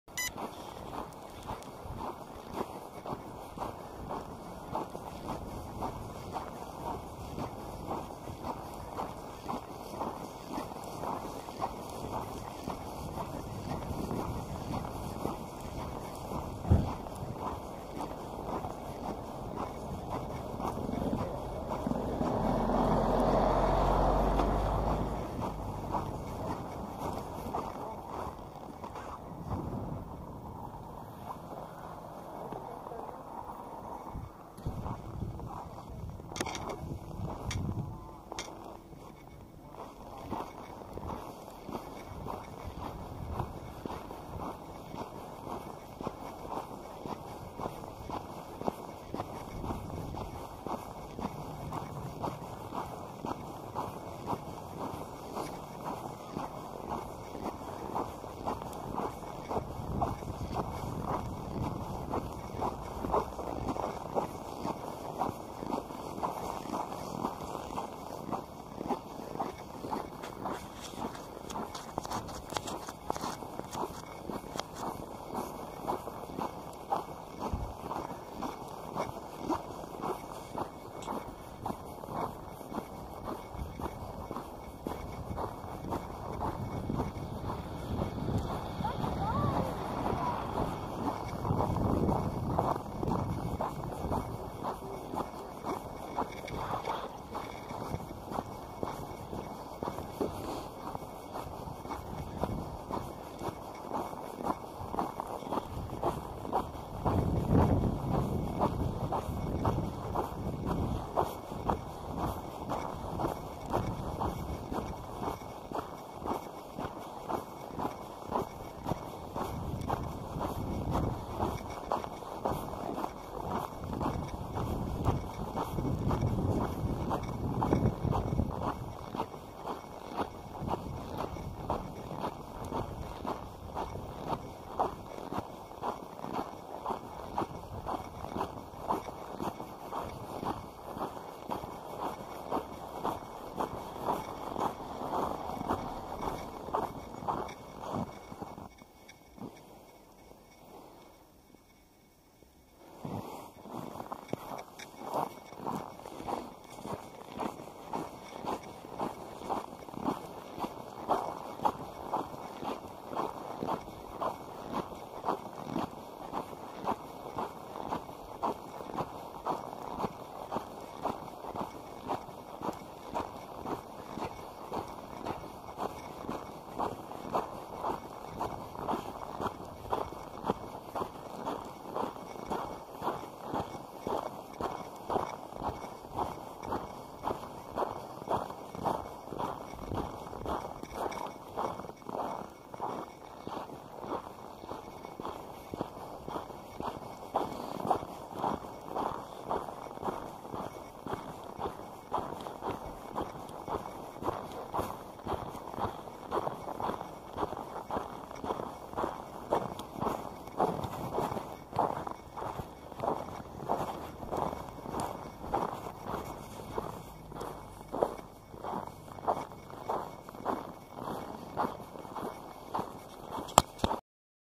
240101 @ Kiruna, Camp Ripan
240101 @ Kiruna in the Swedish Lapland, from Malmfältens Logi to Camp Ripan stroll